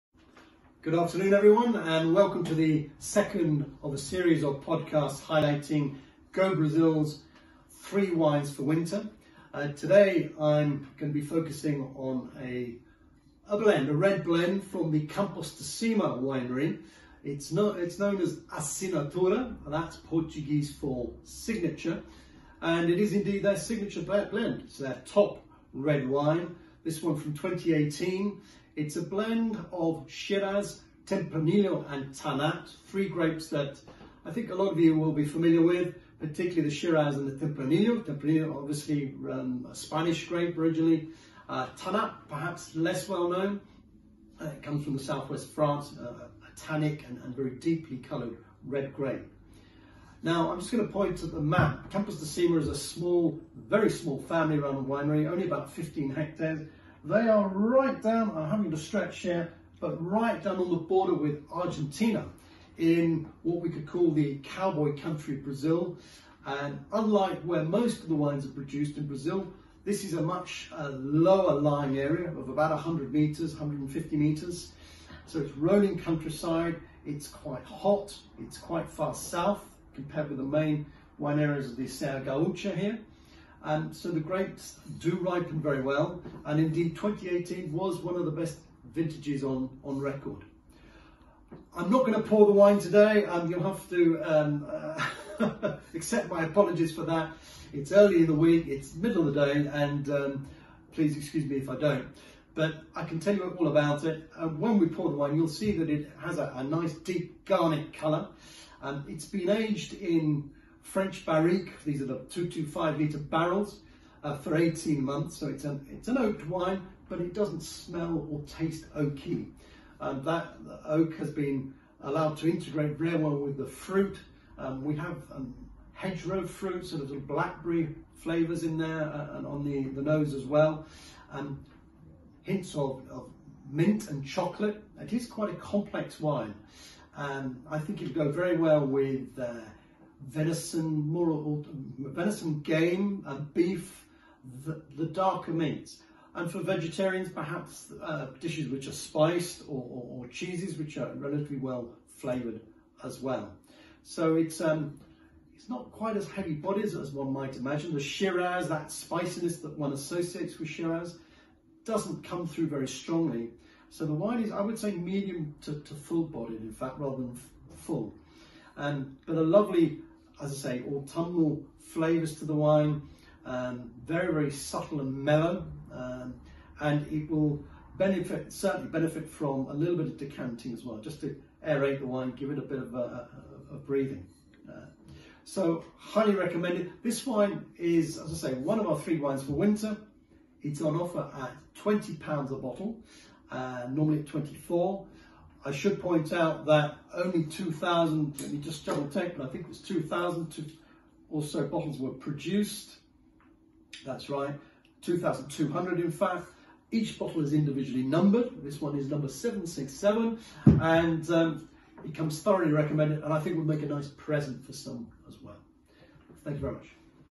Introduction to the Campos de Cima 'Assinatura' 2018 red blend
Go Brazil is proud to present the Campos de Cima winery's signature ('assinatura') red blend, comprising Shiraz, Tempranillo & Tannat grapes, from the 2018 vintage